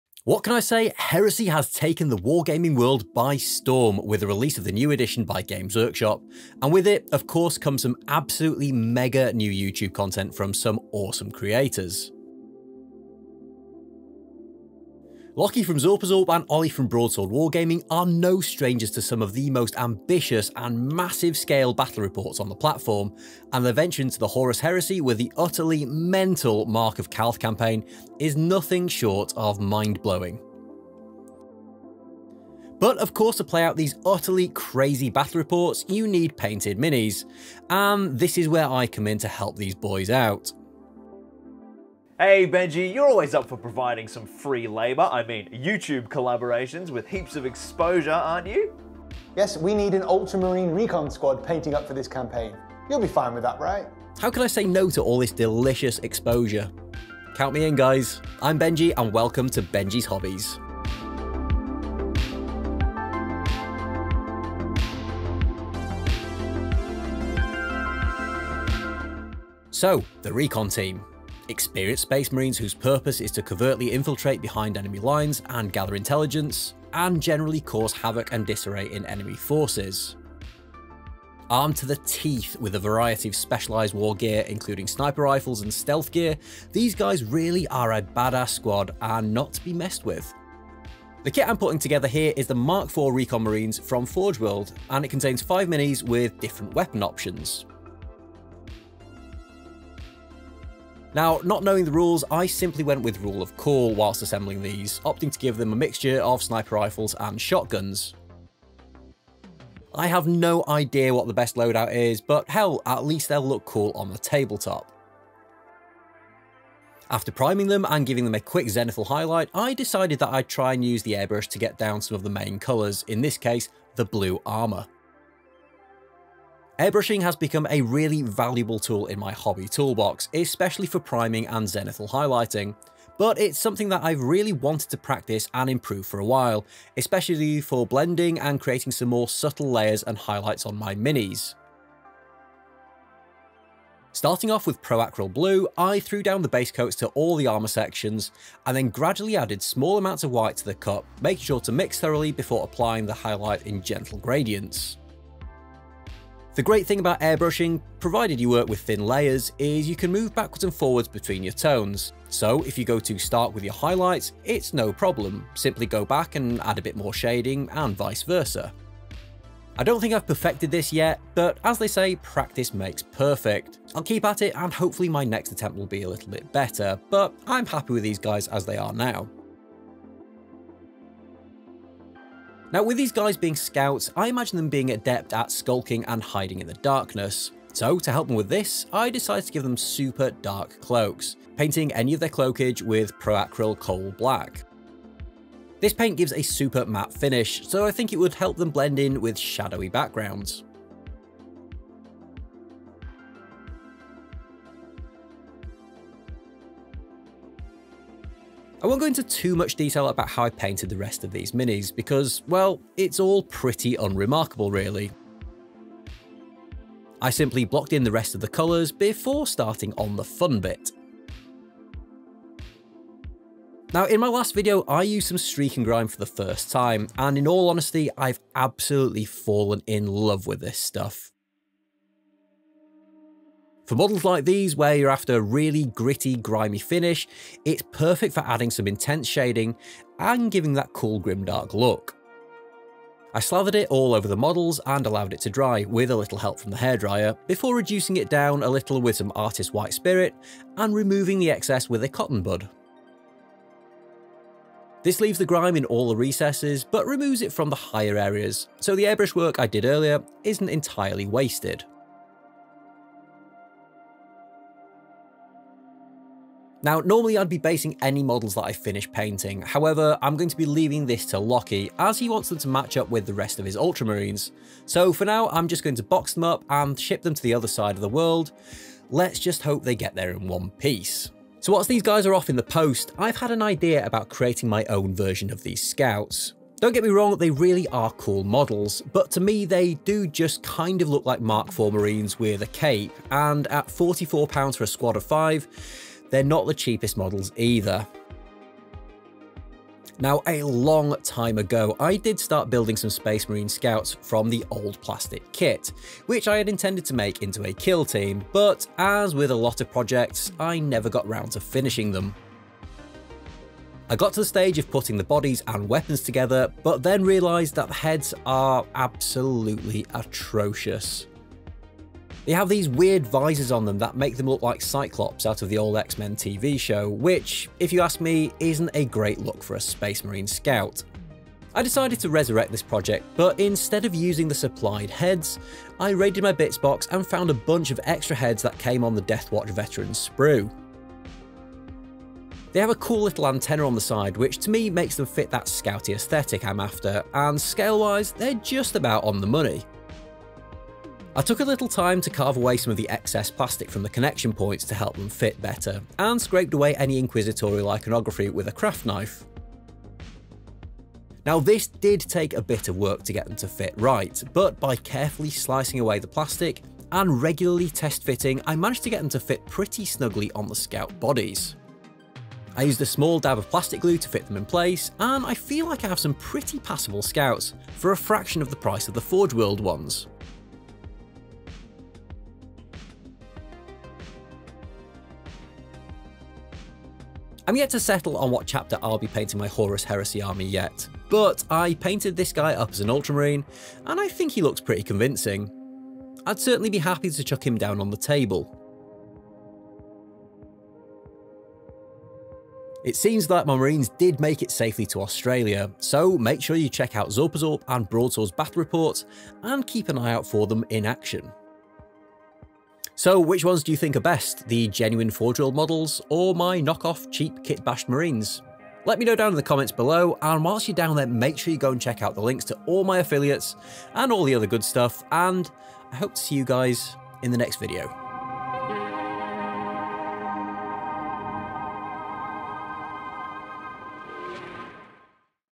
You vs the Space Marines she tells you not to worry about...
I got asked to paint up some Space Marine Scouts to feature in the EPIC Broadsword Wargaming and Zorpazorp Warhammer Horus Heresy narrative campaign - The Mark of Calth! After painting up the Forge World models I decided to try and kitbash my own. Which do you think are best?!